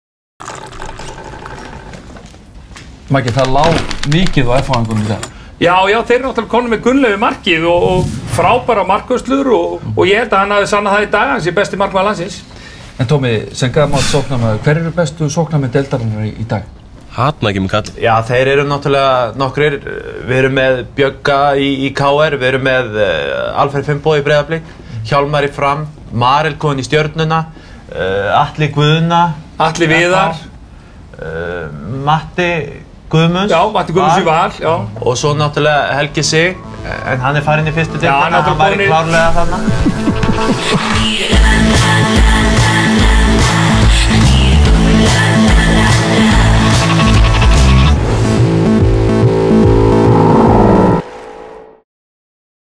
FH stjörnur
Matti Villa er stjarnan í þessari auglýsingu ásamt Gunnleifi Gunnleifs.